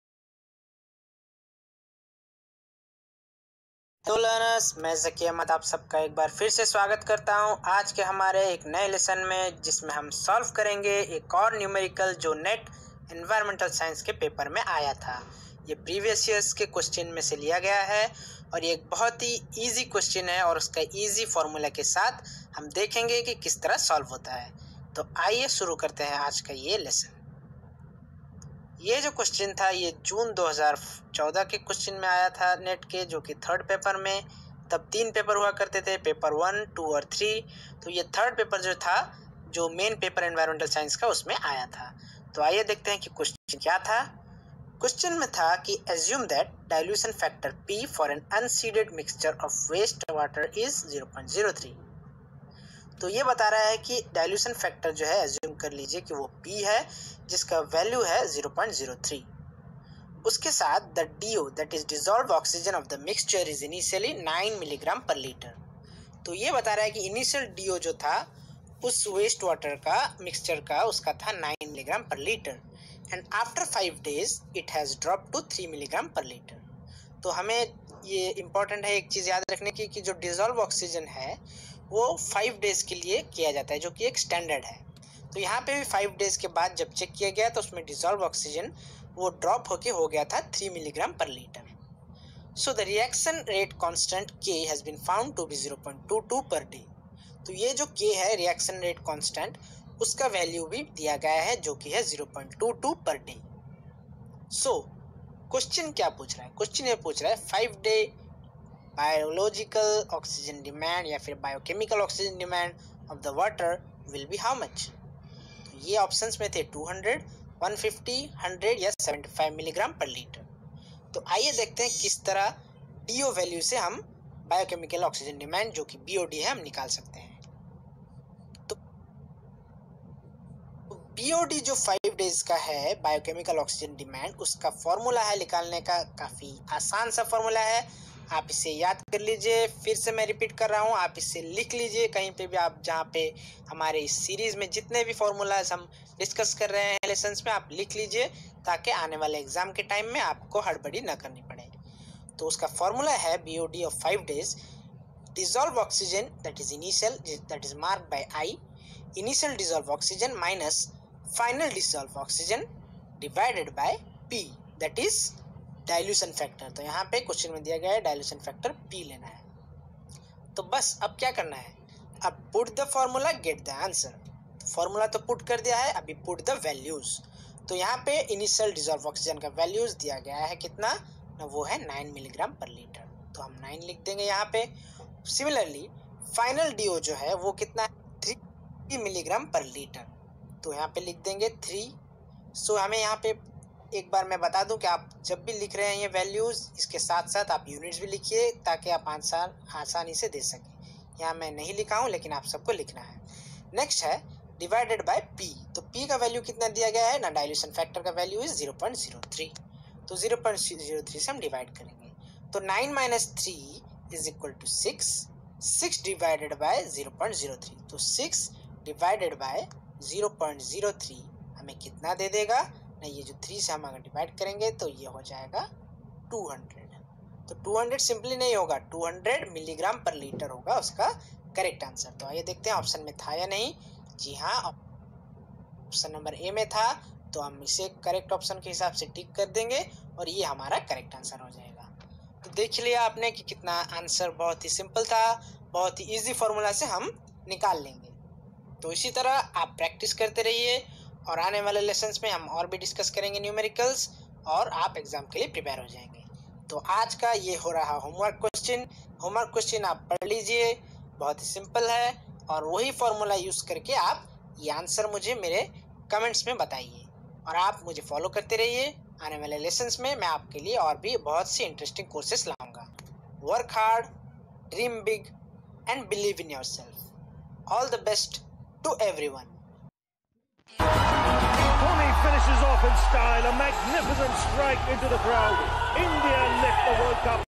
Easy NUMERICAL/B.O.D of Water-NET ENVIRONMENTAL SCIENCE (Part1)
This Lesson deals with NTA UGC NET ENVIRONMENTAL SCIENCE Previous year Numerical- Solving with easy Formula and HOMEWORK QUESTION for Practice..🏆